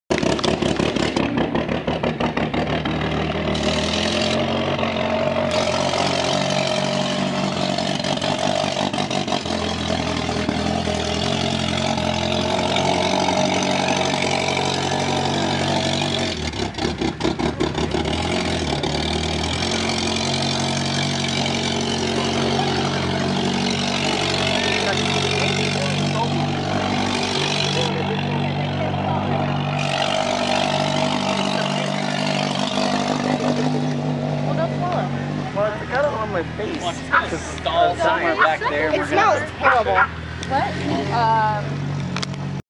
18a Ryan demolition derby
At the fifth annual event dedicated to the fine art of destruction, Destructicon 5 (June 15, 2013).